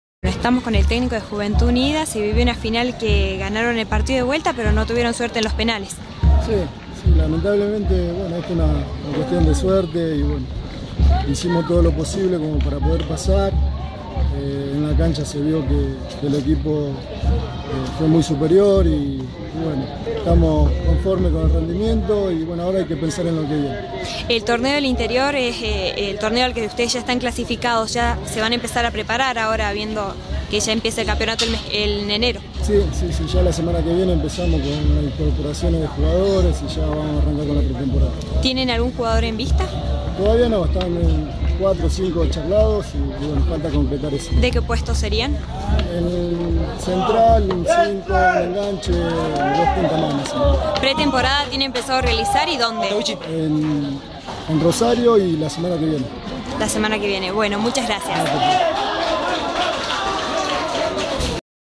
Sergio Albornoz
27/11/2011.- El "Tahuichi" es el técnico de Juventud Unida y nos cuenta la semifinal ante La Merced en la Liga del Valle.